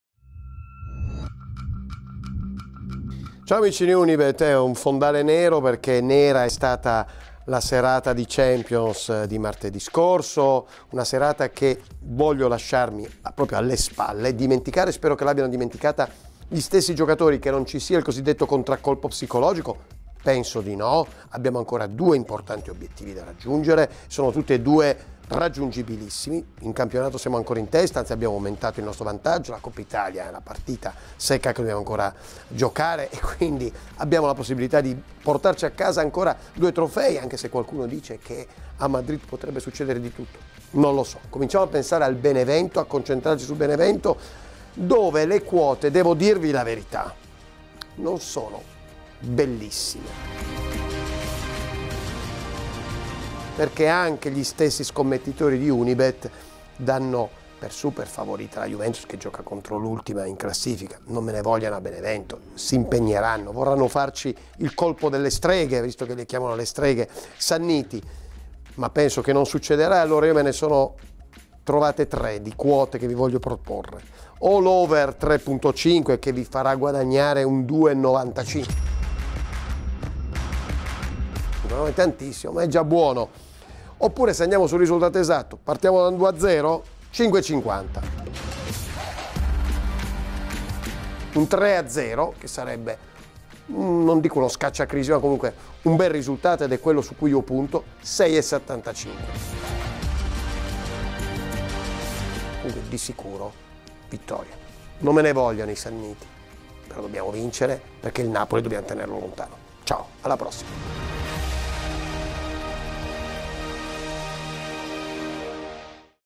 Benevento - Juventus
La Juve, dopo la batosta con il Real, vuole mantenere le distanze sul Napoli in campionato e la vittoria a Benevento è d'obbligo!
Marcello Chirico prevede un 3-0 per i bianconeri al Vigorito...